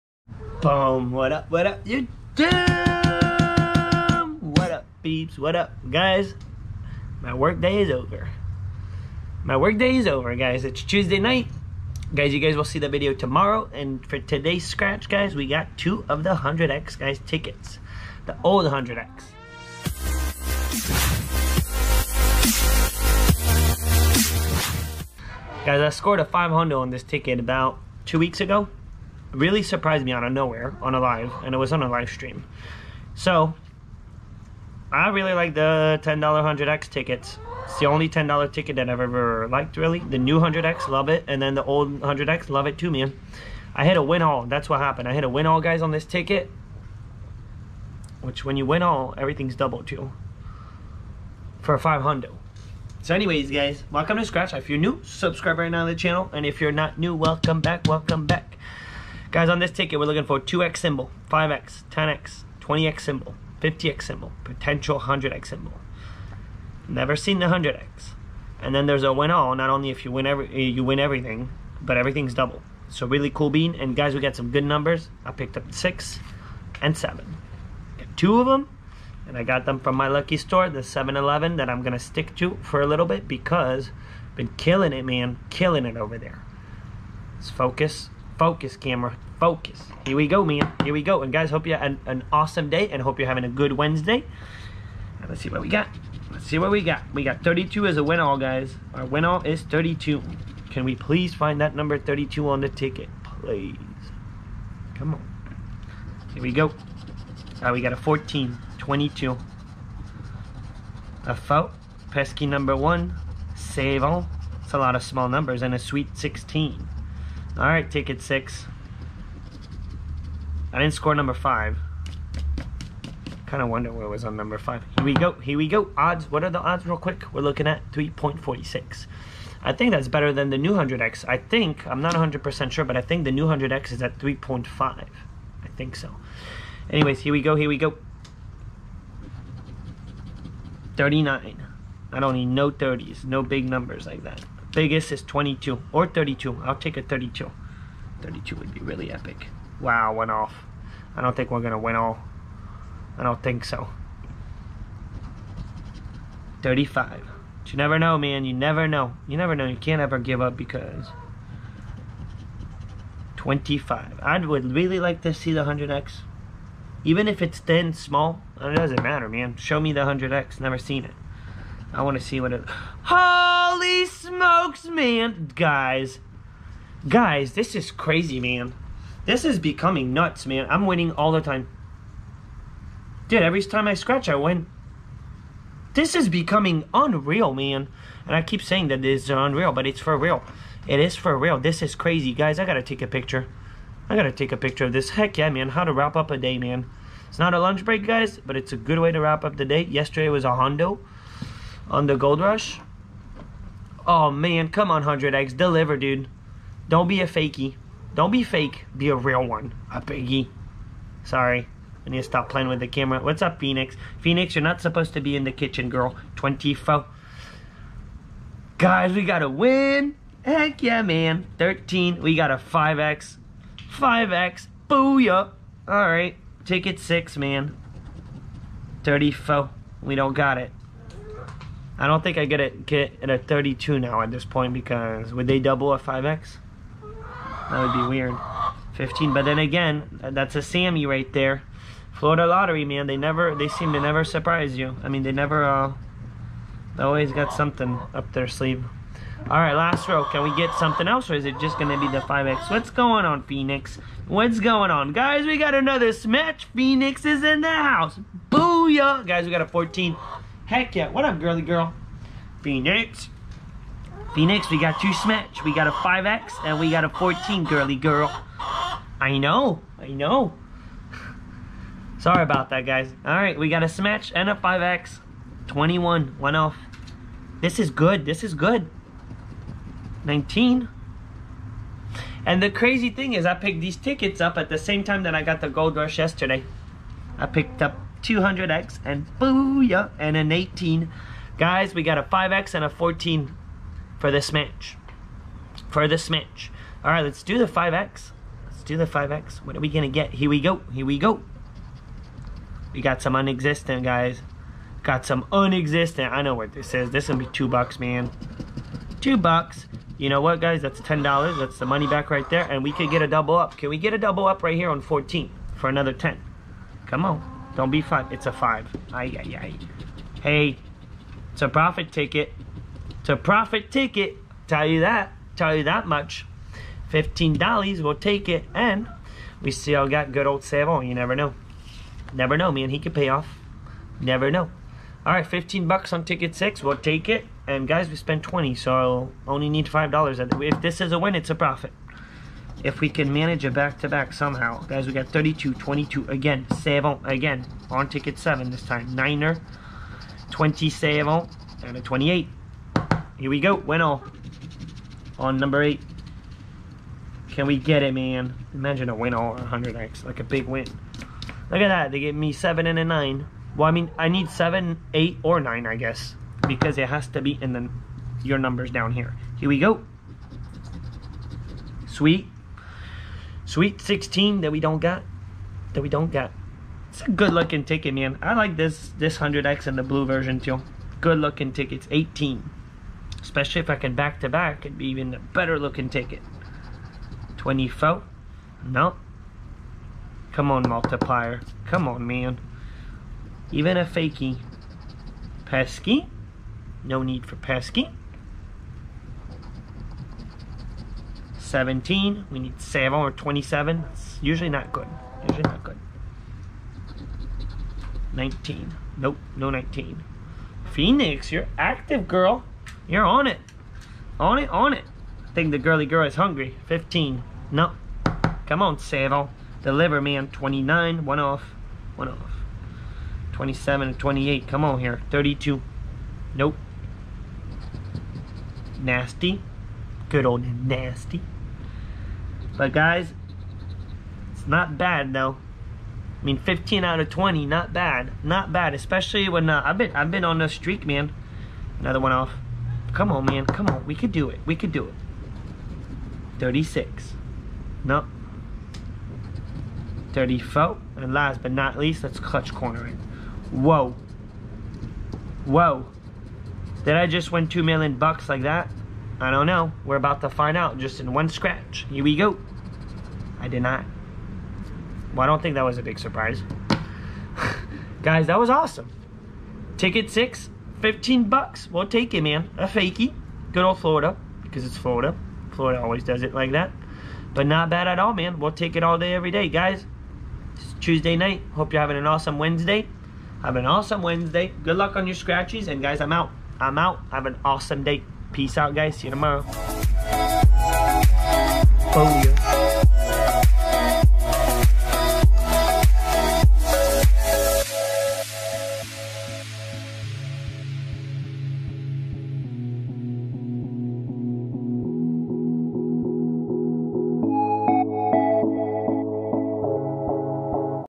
The 100 Times Ticket is Where Its At!!!
Bought 2 of the 100X scratch off ticket from the florida lottery and found a 5X symbol!!!Happy Thursday Guys!